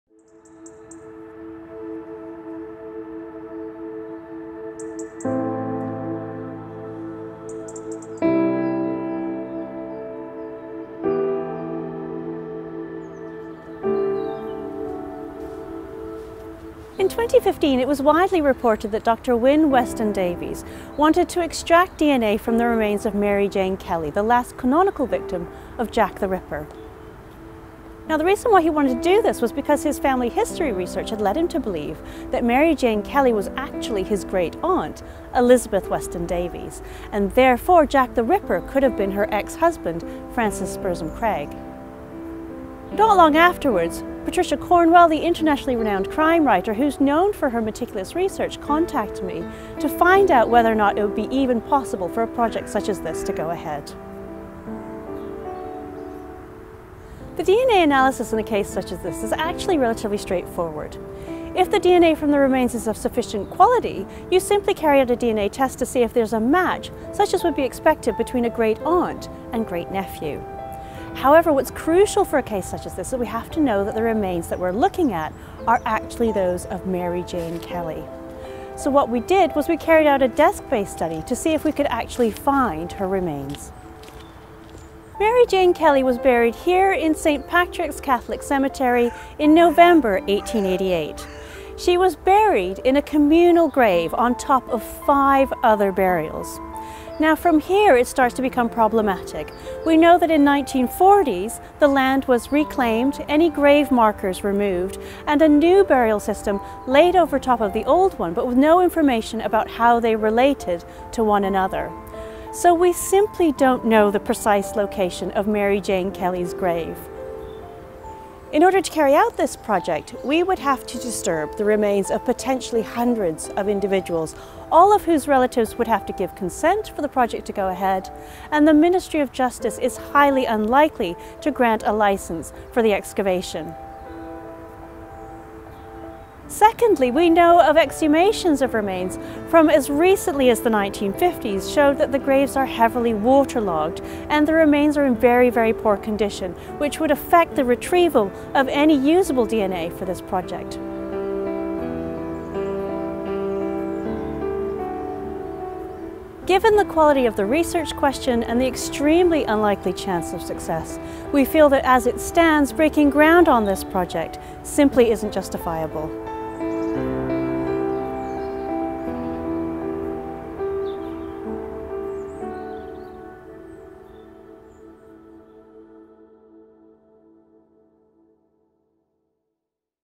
Mary Jane Kelly's Grave - Victim of Jack the Ripper
To this day, Mary Jane Kelly, the fifth canonical victim of the Victorian serial killer known as Jack the Ripper, remains a much-discussed but enigmatic figure. Her brutal murder on 9th November, 1888 shocked a nation already profoundly horrified by the spate of ghastly murders that had taken place that year in Whitechapel, in London’s East End. Hundreds of people turned out to pay their respects when she was buried at St Patrick’s Catholic Cemetery in Leytonstone ten days later. Yet, despite her notoriety, there is so little verifiable information known about her, in stark contrast to the Ripper’s other four victims, that some authors have concluded that she was living under an assumed identity, leading to speculation about who she truly was.

In 2015, a new book purported to reveal Kelly’s true identity. The author, Wynne Weston-Davies, claimed that the woman known to everyone as Mary Jane Kelly was living under a pseudonym and was in fact his great-aunt Elizabeth Weston Davies. Subsequently, Dr Turi King of the University of Leicester was approached by the author Patricia Cornwell regarding the putative testing of DNA from the remains of Mary Jane Kelly and matching them against those of Wynne Weston-Davies, with whom she had been in contact.

Following discussions with all parties, in early 2016 it was agreed that a desk-based study would be produced by a team of researchers from the University of Leicester to address three key questions: (1) What would be required to determine a DNA match between Wynne Weston-Davies and the remains of his great-aunt, Elizabeth Weston Davies, the individual putatively buried under the alias Mary Jane Kelly? (2) What was the likely condition and survival of her remains, if an exhumation was carried out? And (3) Where was Mary Jane Kelly buried and could her grave site be accurately located using surviving records? All three questions would need to be resolved if an application for exhumation was to be submitted to the Ministry of Justice.

This report is prepared based on information current and available as of February 2017. A site visit to St Patrick’s Catholic Cemetery, Leytonstone was undertaken by the Research Team in May 2016 to examine the burial area and carry out research in the cemetery’s burial records. Additional research examined the case for Kelly being Elizabeth Weston Davies.

The project has concluded that whilst DNA testing of the remains of Mary Jane Kelly would allow for a comparison to be made between those remains and Wynne Weston-Davies, in order to determine if the genetic data is consistent with them being related; at present, much of the case for Mary Jane Kelly and Elizabeth Weston Davies being the same individual is circumstantial or conjectural, with no conclusive evidence that the two are the same person.

Today, Kelly’s modern grave marker likely has little relevance to the real location of her grave, which appears to have been unmarked before the 1980s, and the present marker is positioned in a burial system that has only been in use since the 1940s. No evidence for the original 19th century layout of graves can be seen on the ground but two hypothetical grave areas and a search area of c.85 sq m has been generated by reconstructing the 19th-century burial layout from burial registers and historic maps. However, this area likely contains between 54 and 394 sets of human remains, from a potential 150 to 1,240 named individuals. Genealogical research would take years to trace present-day relatives for these individuals, all of whom would probably have to give their consent to any exhumation. Furthermore, ground conditions in the cemetery do not appear to be favourable for the good preservation of coffin material, and any surviving skeletal remains are likely to also be in poor condition. All said, the number of unknown variables mean that there is still no guarantee that Mary Jane Kelly is buried within the hypothetical search area, and unfortunately, even if she is, it is very likely that her grave has been disturbed or destroyed by more recent grave digging.

As information presently stands, a successful search would require a herculean effort that would likely take years of research, would be prohibitively costly and would cause unwarranted disturbance to an unknown number of individuals buried in a cemetery that is still in daily use, with no guarantee of success. As such, it is extremely unlikely that any application for an exhumation licence would be granted.

This film was produced by Marketing and Student Recruitment at the University of Leicester.

Filmed and Edited by Carl Vivian
Produced by Dr Turi King